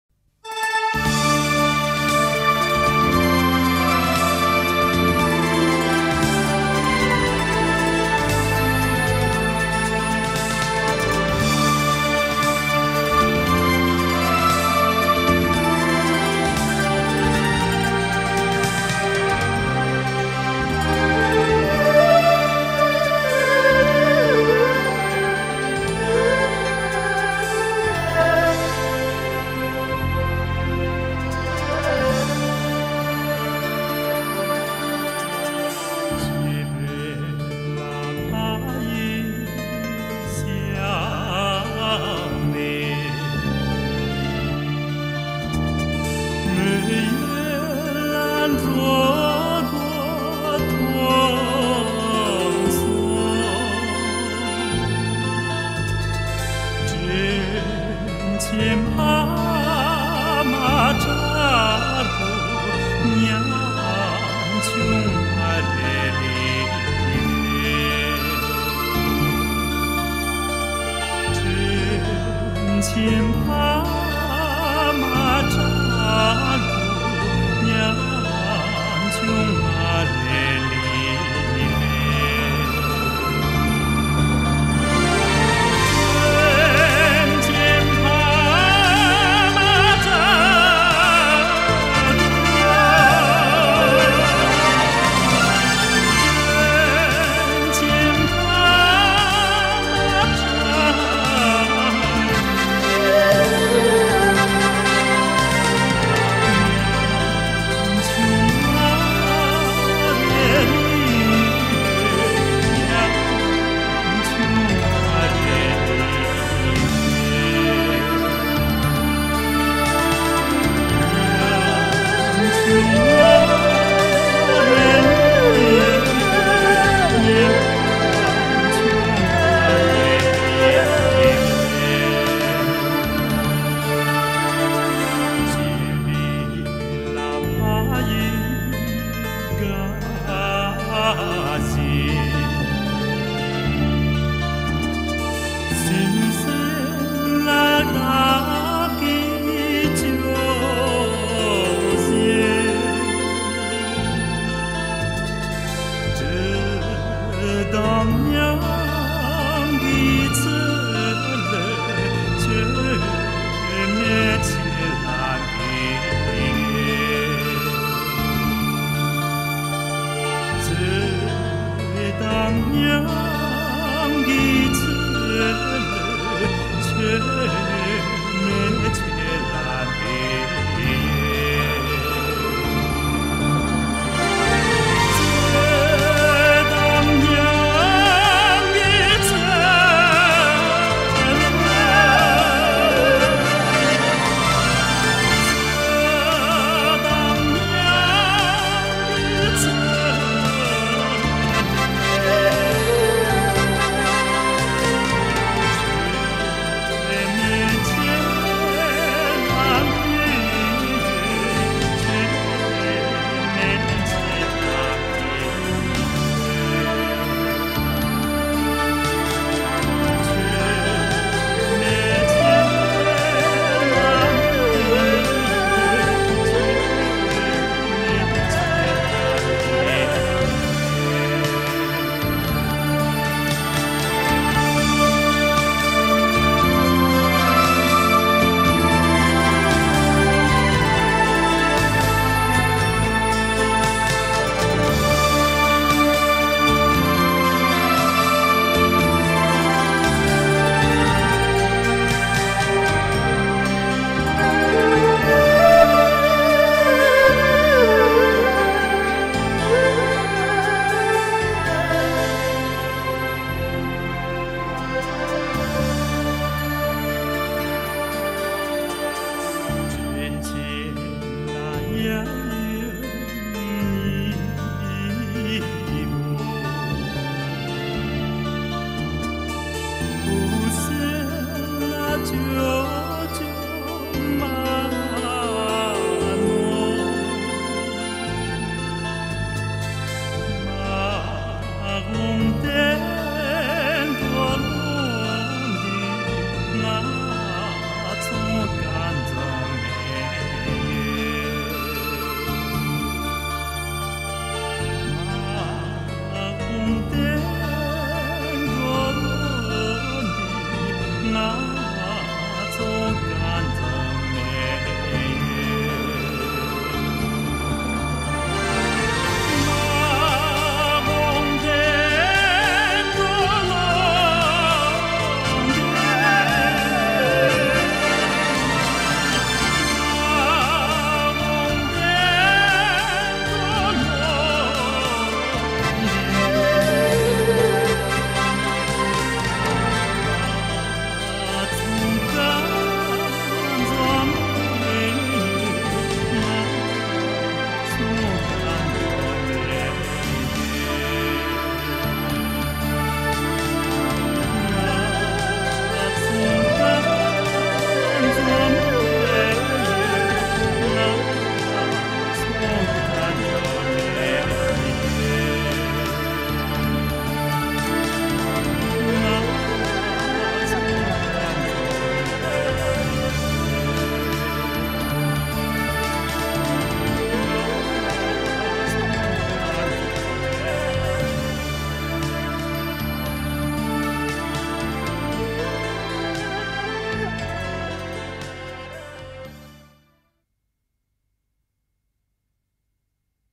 Tibetan Culture flourishing (Tsegoen) #552
Actually Tibetan culture is flourishing now. So many singers, dancers, composers, and writers are growing up. Although there is contradiction between tradition and modernization, this is what every country has experienced.